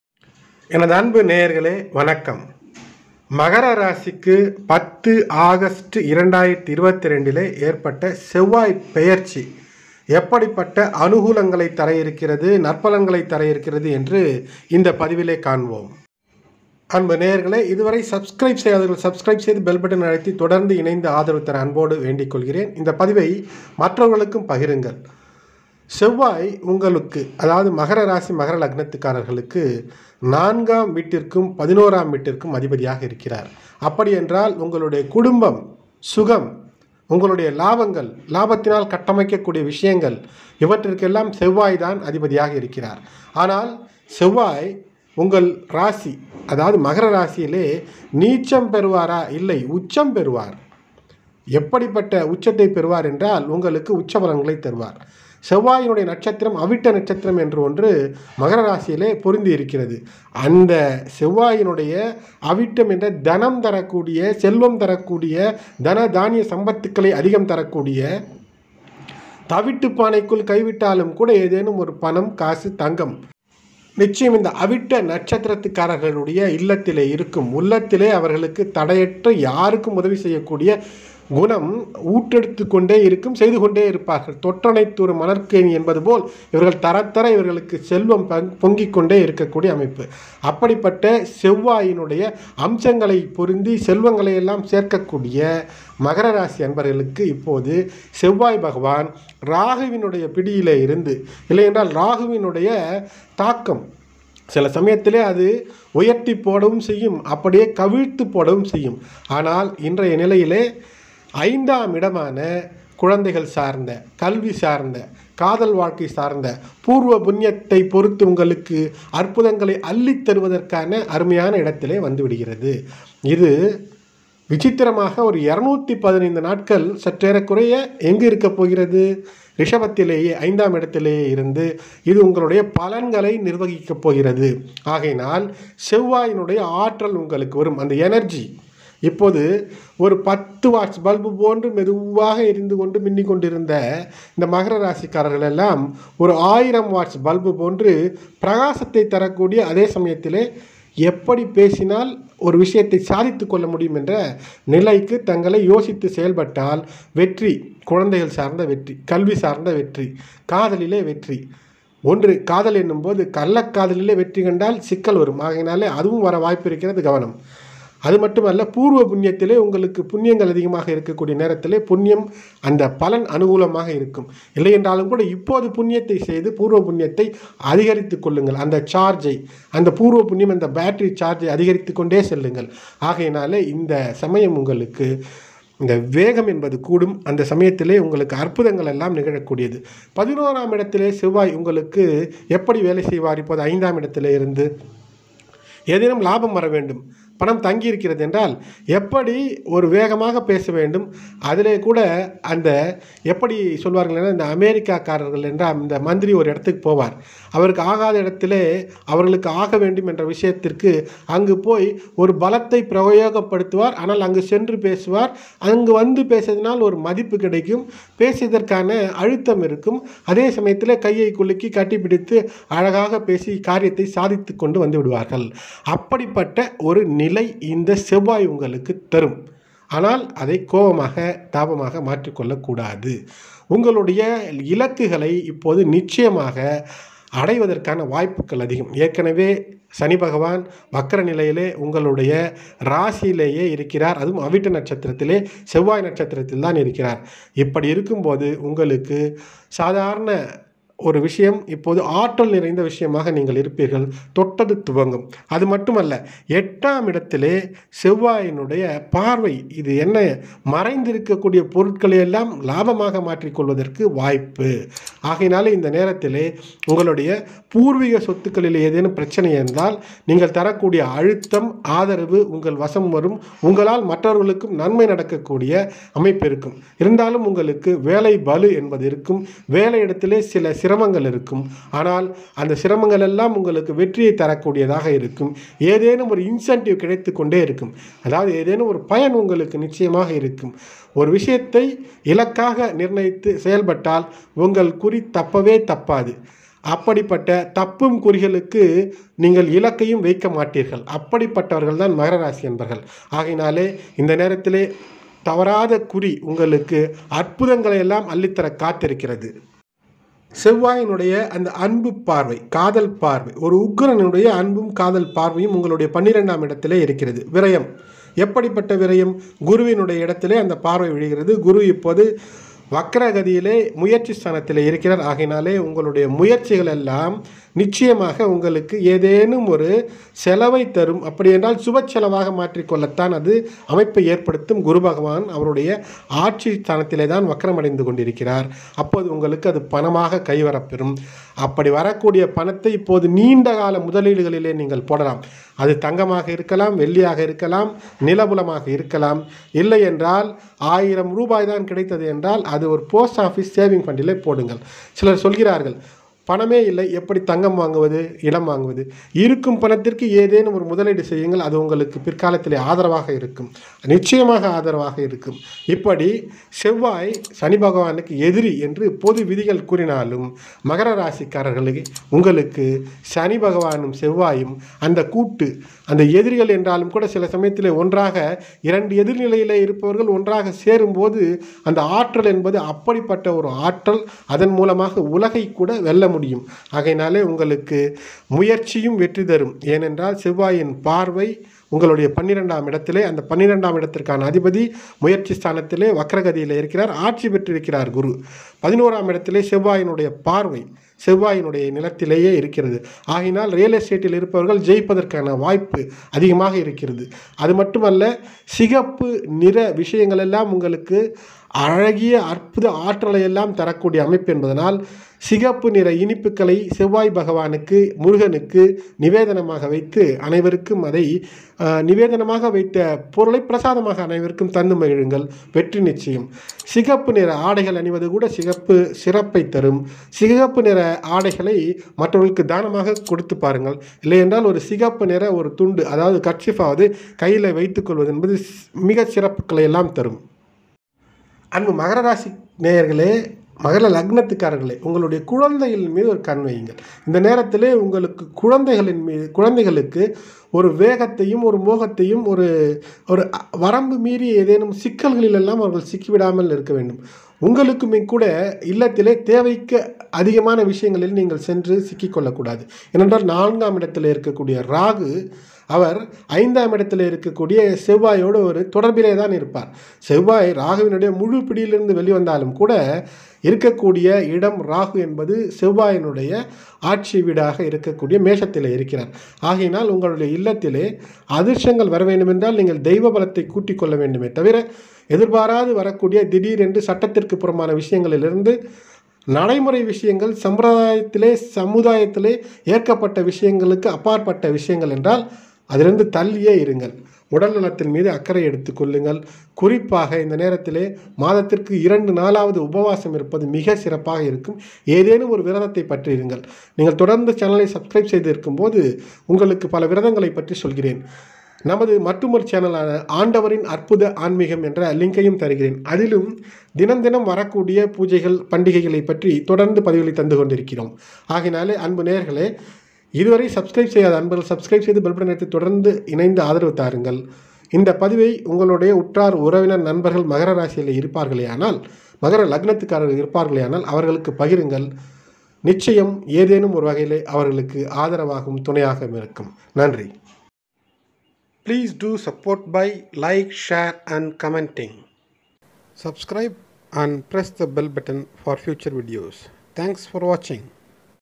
Makaram - Sevvai peyarchi August 2022 - மகர ராசி ஆகஸ்ட் 2022 செவ்வாய் பெயர்ச்சி பலன்கள் -♑Capricorn
Mars Transit in Taurus 2022
குருப்பெயர்ச்சி பலன்கள் 2022 – தனுசு, இன்றைய ராசிபலன், தினசரி ராசிபலன்.
ஆகஸ்ட் மாத ராசி பலன் 2022

Usage of the opinion expressed in the video is at your sole discretion and the channel should not be help responsible for any of the outcomes.